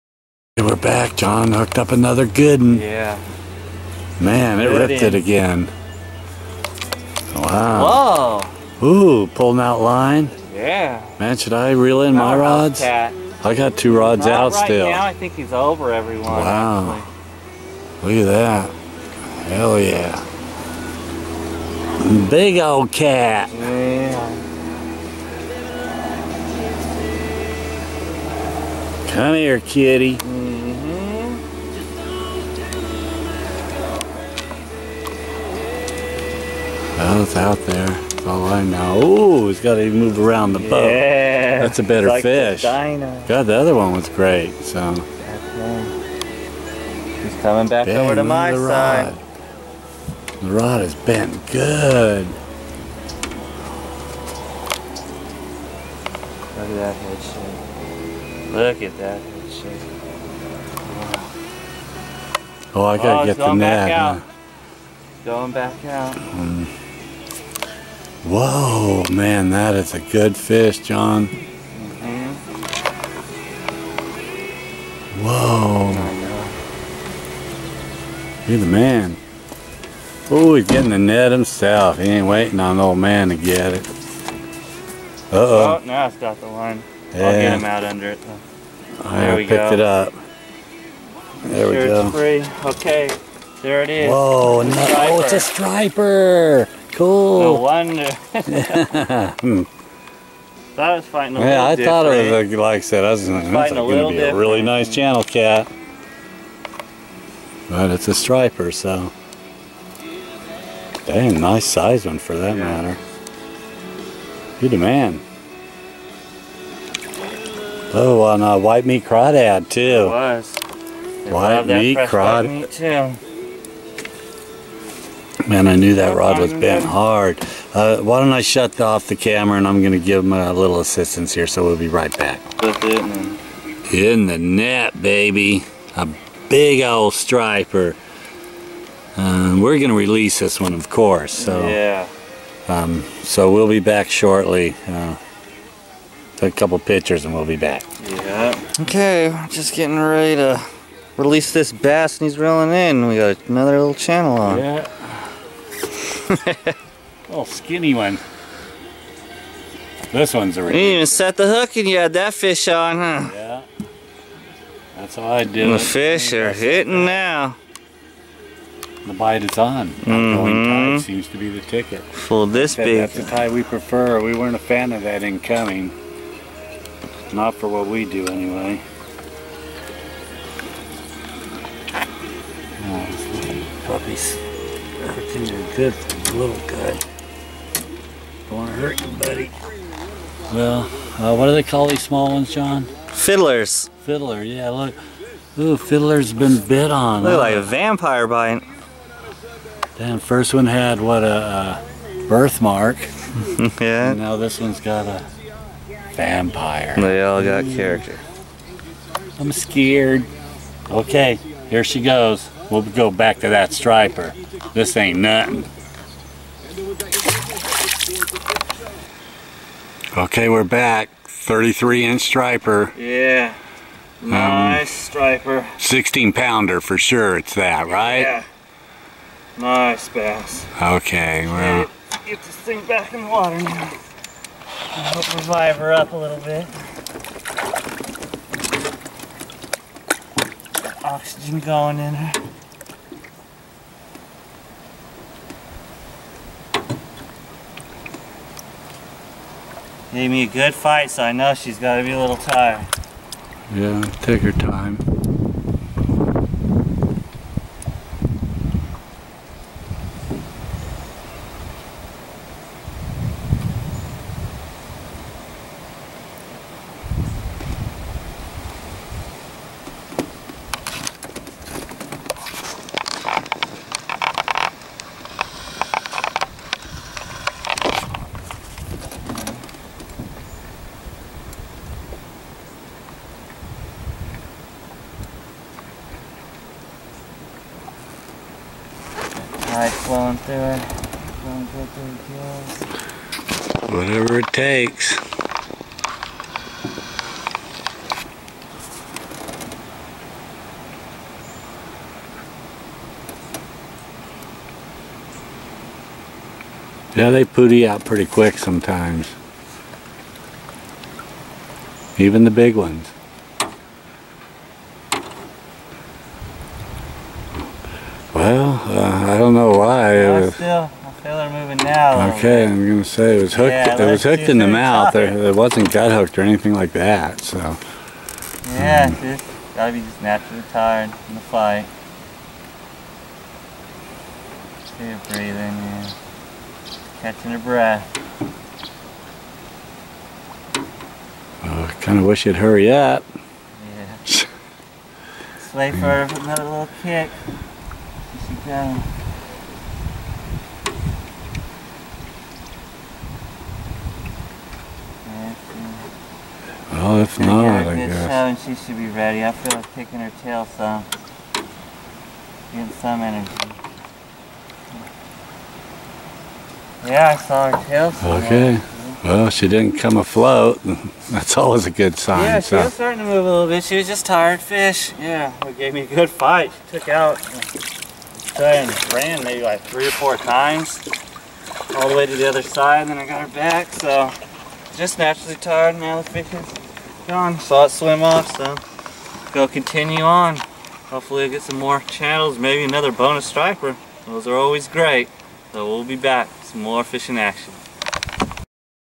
BIG STRIPER ON CRAWDAD ! .... 4-6-14
WHILE CAT FISHING ..YOU SOME TIMES GET A BONUS FISH. THAT WAS THE CASE TODAY THIS BASS HIT ON A CRAWDAD TAIL ..ALSO HAD ANOTHER ONE THAT ALMOST TOOK THE ROD IN WATER ...ON THE CALIF. DELTA